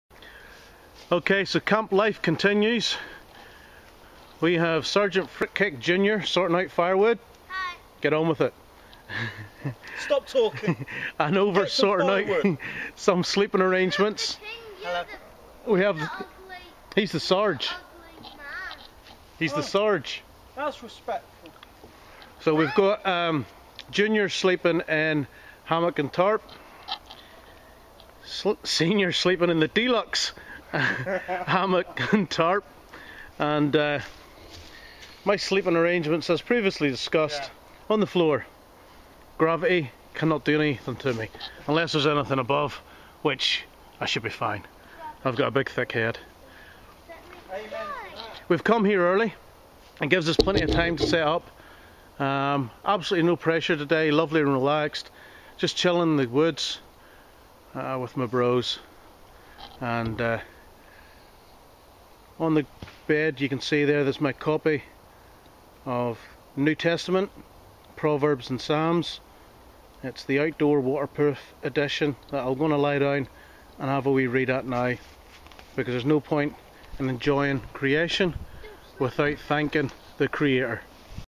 Wildcamp with SgtFruitcake & Sgt Jnr
and the madness continues.
but in the midst of the chaos taking time to be thankful.

ignore the pecking order of sleeping arangements I stayed warm all night unlike my companions